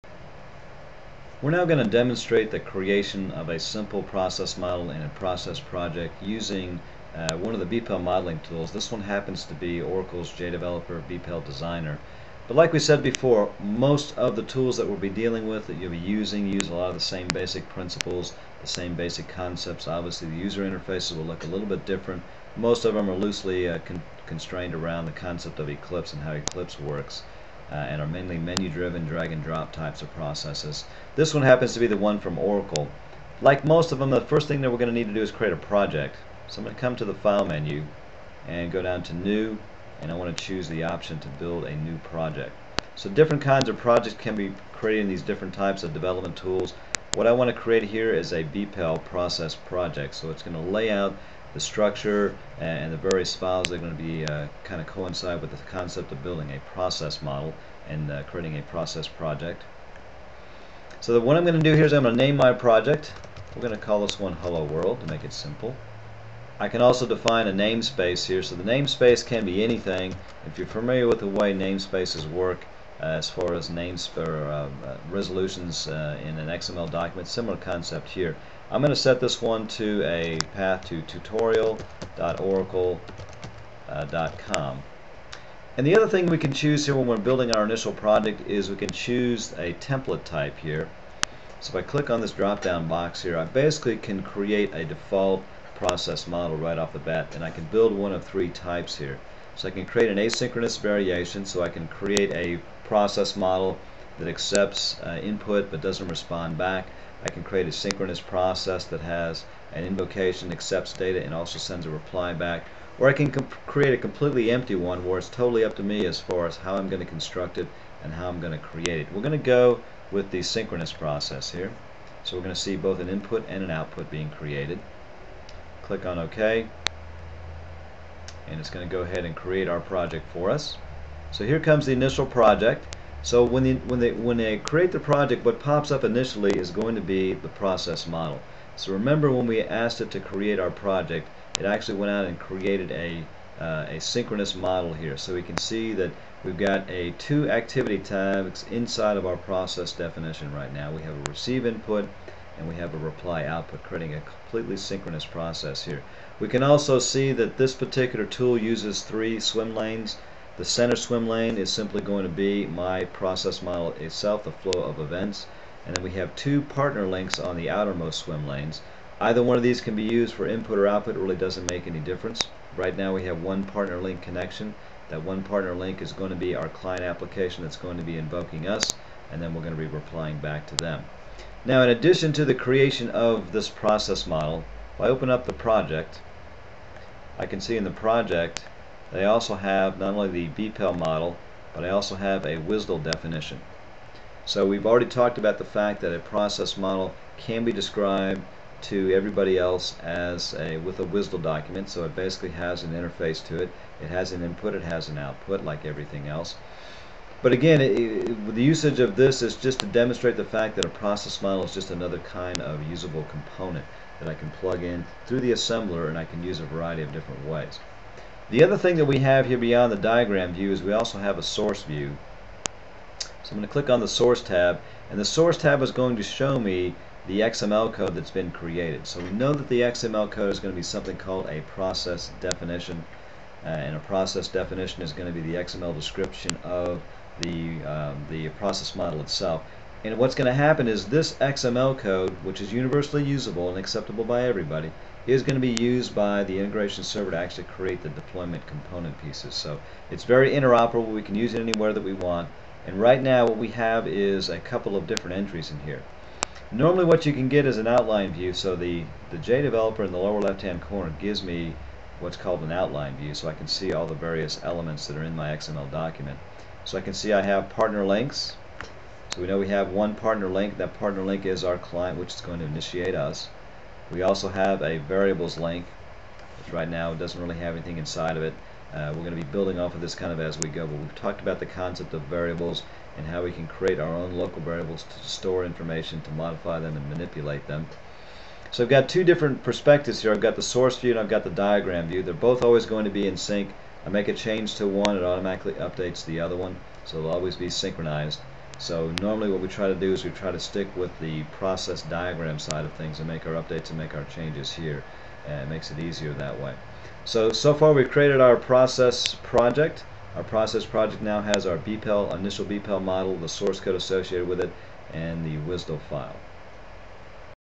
Step1-HelloWorld Project Creation
Using the SOA Suite in Oracle to demonstrate the creation of a BPEL project